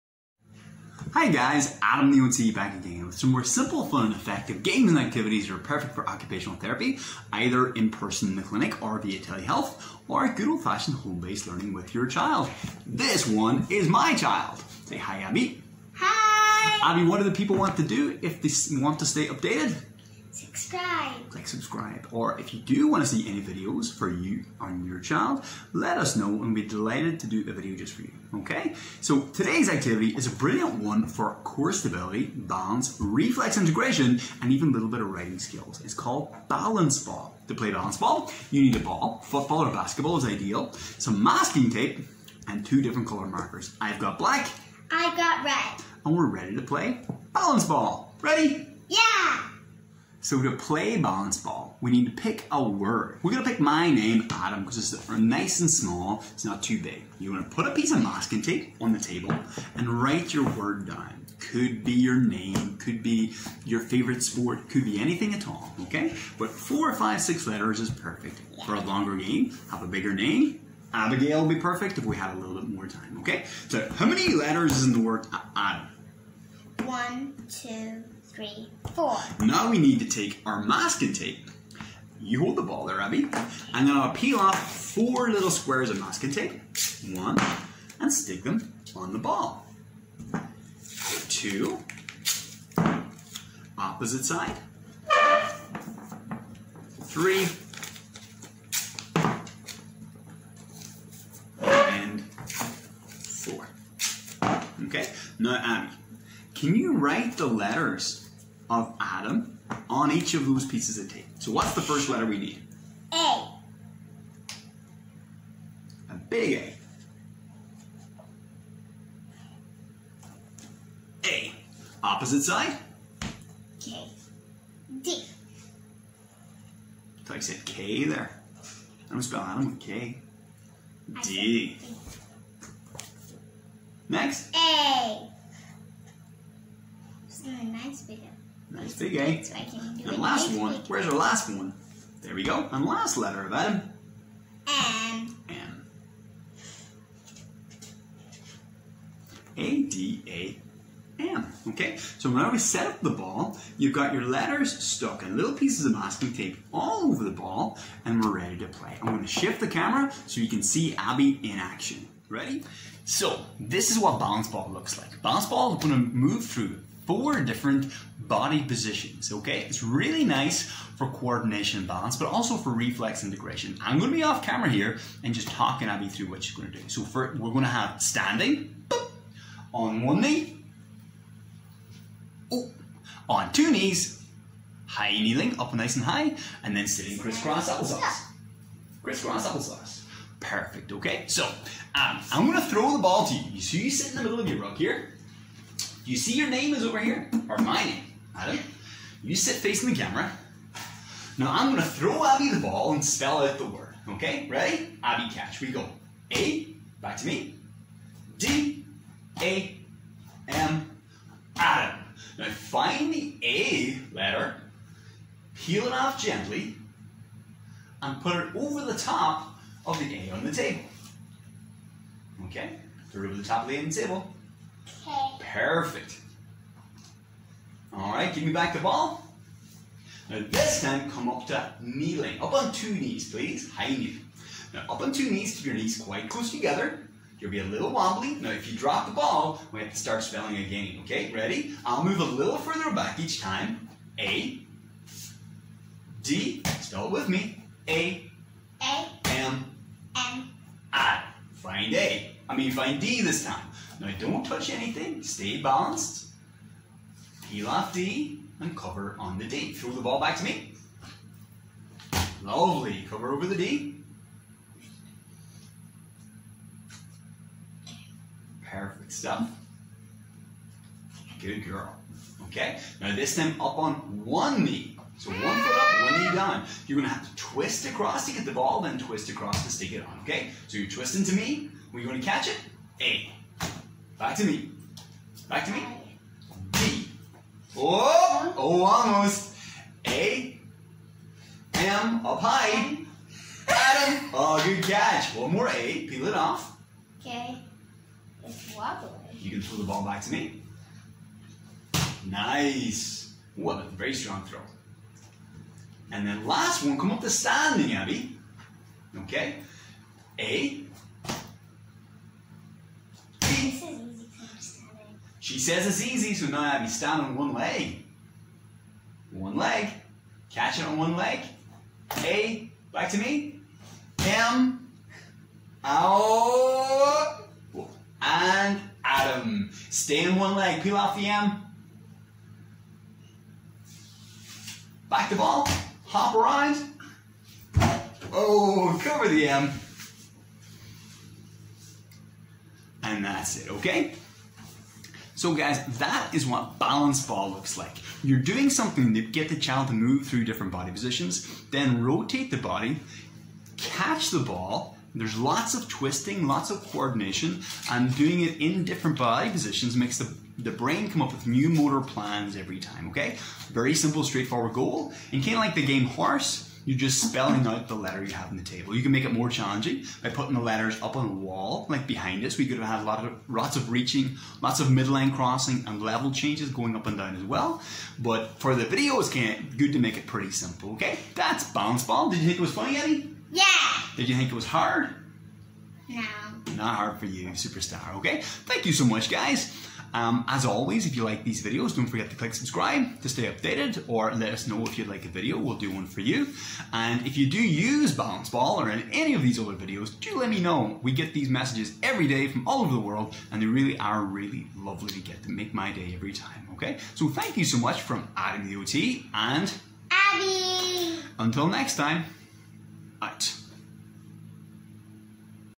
Body Position Balance Ball 🏀 A Reflex Integration, Balance & Stability Game for OT & Home Learning
Using only a ball and some tape this super simple and fun activity helps your child develop their writing, fine motor, balance and postural stability while working through a number of different body positions designed to support the development of coordinated, integrated whole body movement 🕺

By changing the childs posture from sitting to one-half kneeling (one knee/one foot), tall kneeling and standing we continue to challenge the child's motor coordination while keeping the same simple, clear and motivating goal throughout 😄

For additional challenge they adding in level changes, use a weighted ball, bring in positions such as side sit (mermaid) or long leg sitting with a greater rotation during the activity.